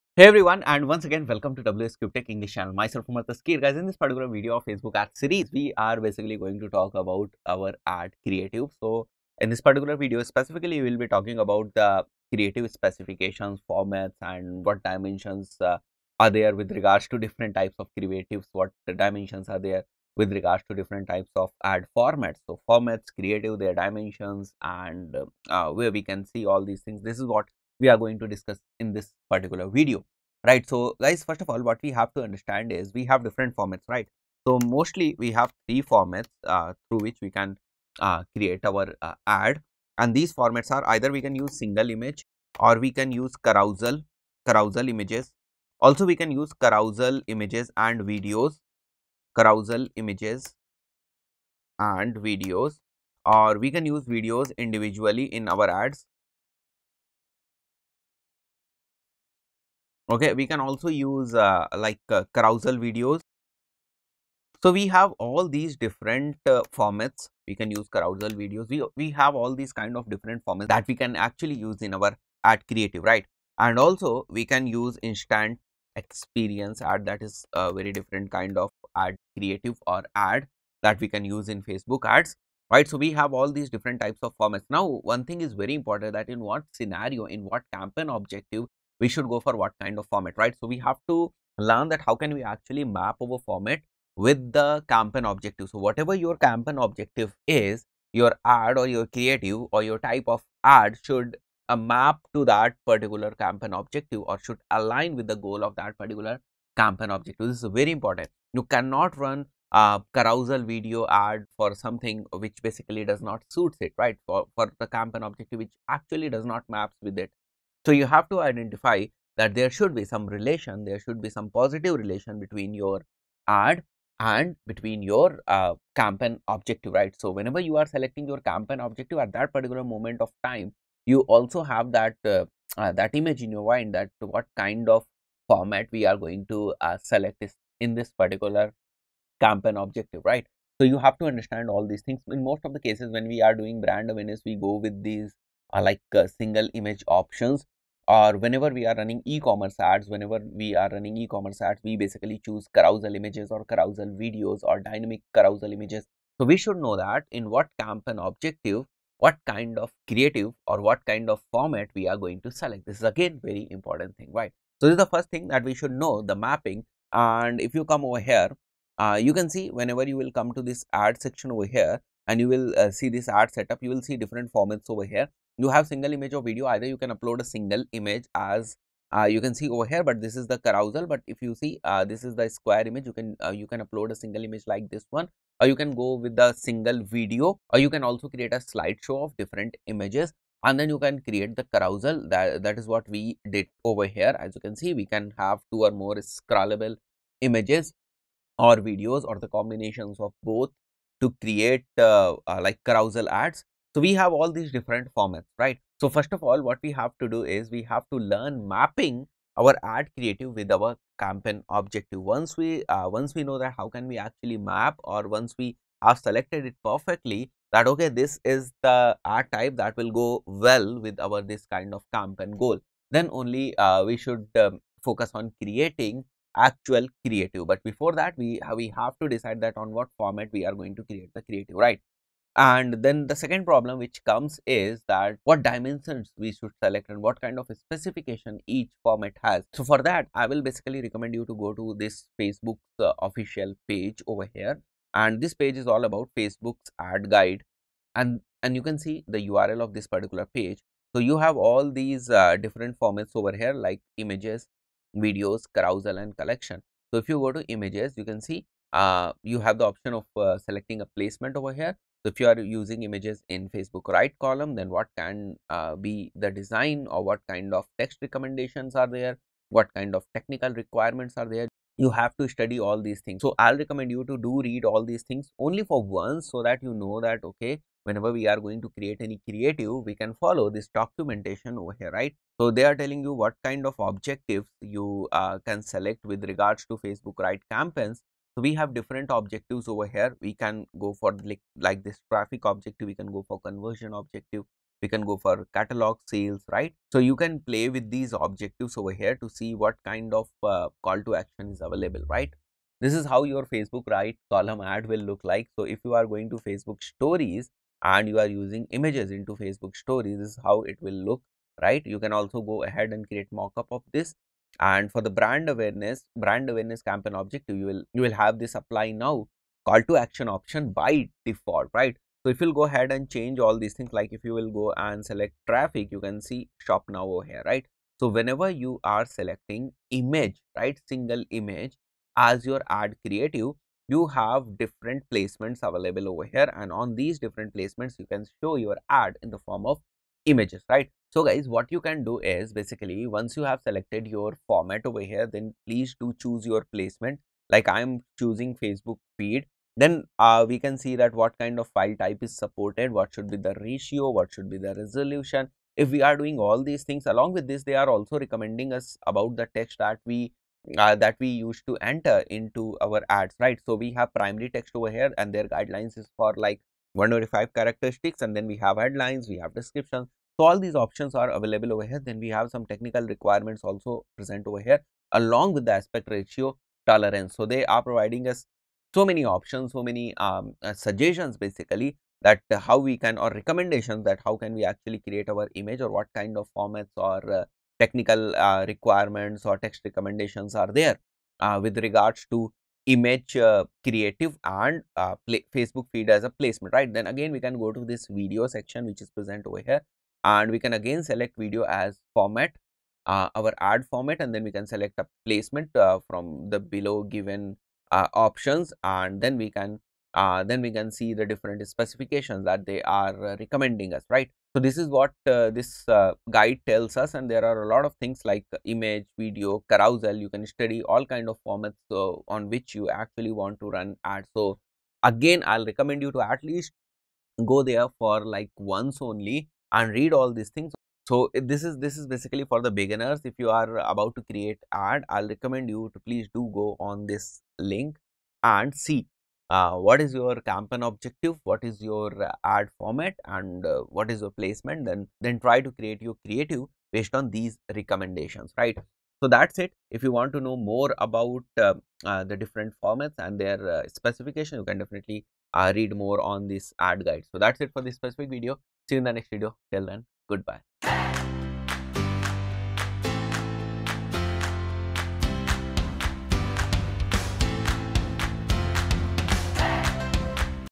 Facebook Ads Course - Different Types of Facebook Ads & Ad Formats | Types of Facebook Ads (Part-22)
In this video, I am trying to explain Different Types of Facebook Ads & Ad Formats (in English). Please do watch the complete video for in-depth information.

✔ Offering Job Oriented Most Latest, Updated and advanced Digital Marketing Courses with Practical, Hands-on Live Projects Training & Exposure. ( Both in English & Hindi)